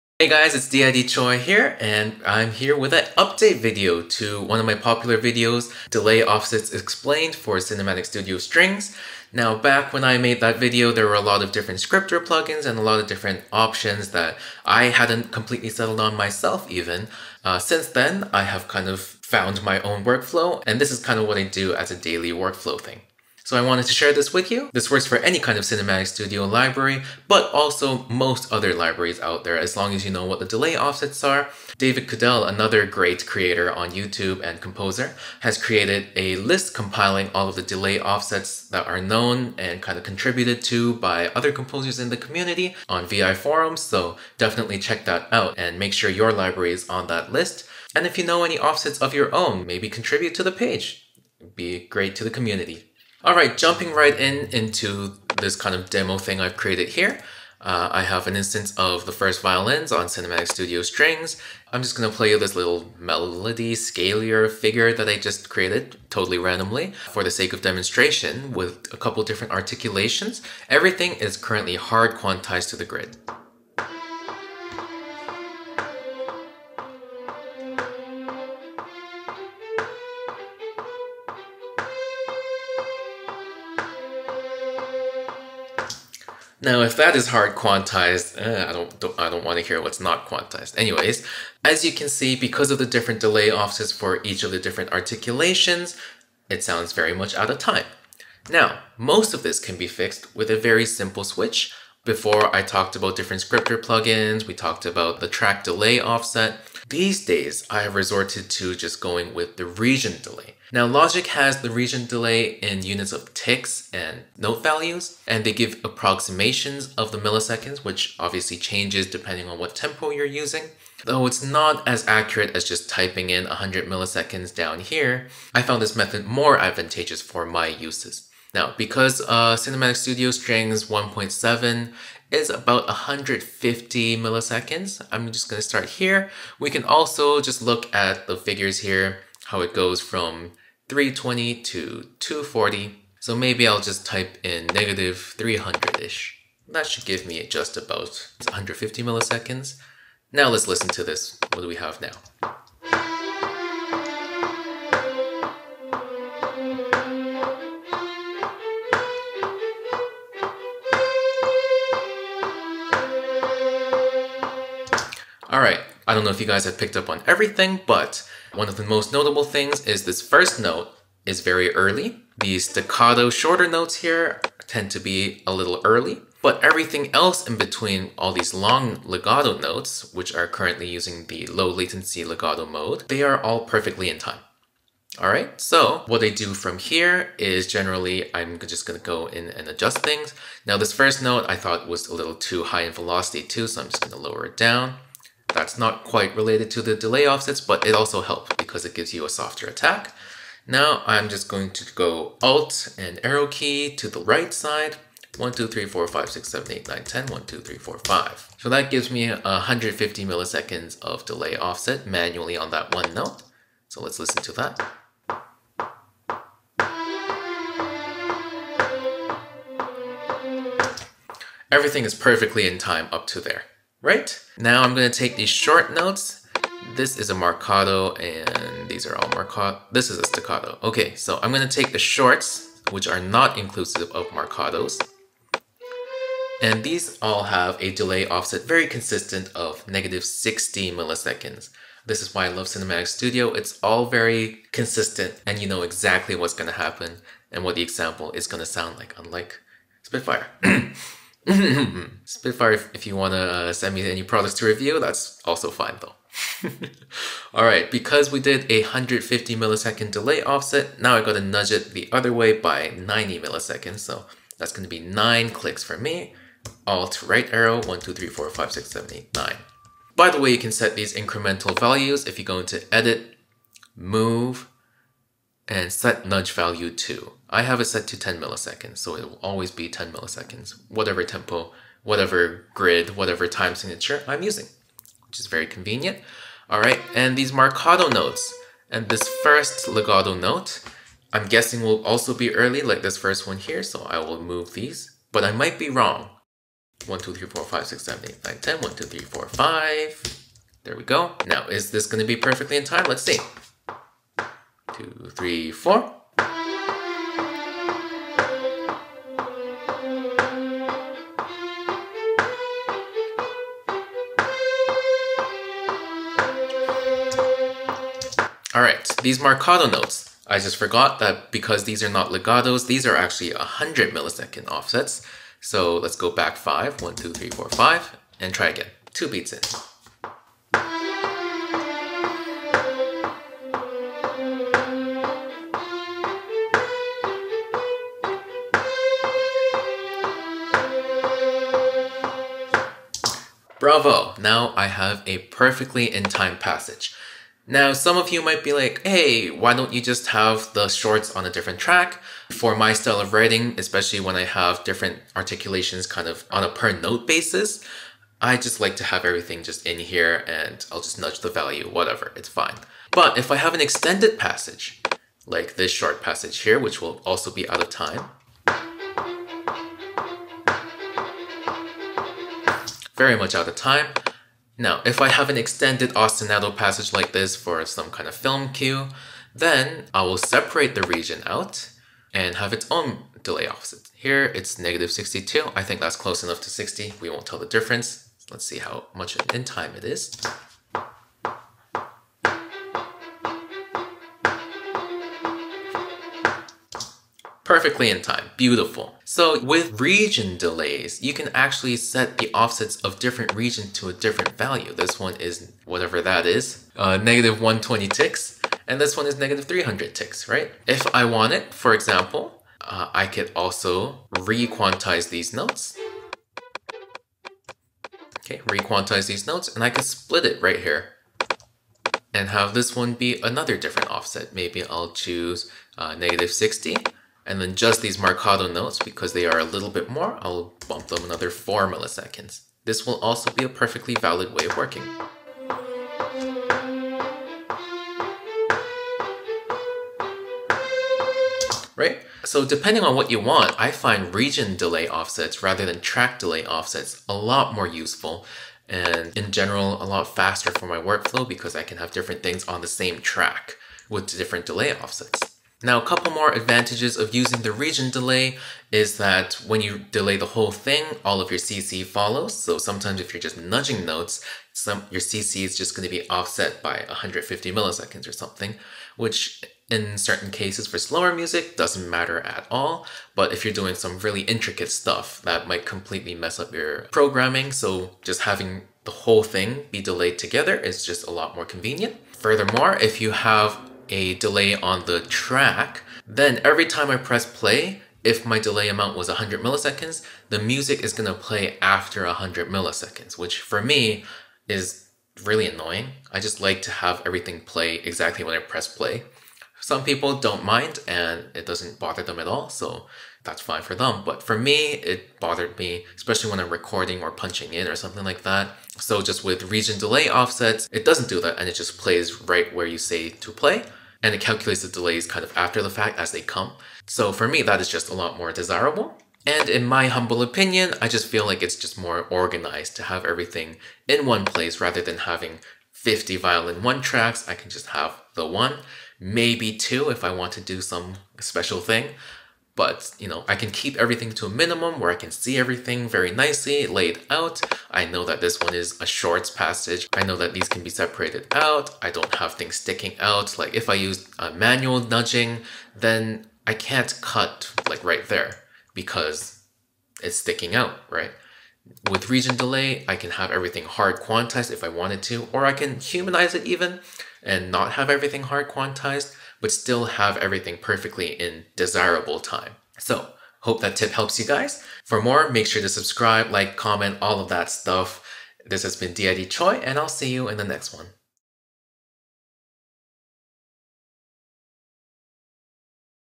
Cinematic Studio Series DELAY OFFSETS | UPDATED 2023
Follow up on my summary of Delay Offset solutions for Cinematic Studio Strings. I think track offsets are much more commonplace now. Here is how I generally tackle the issue in my workflow.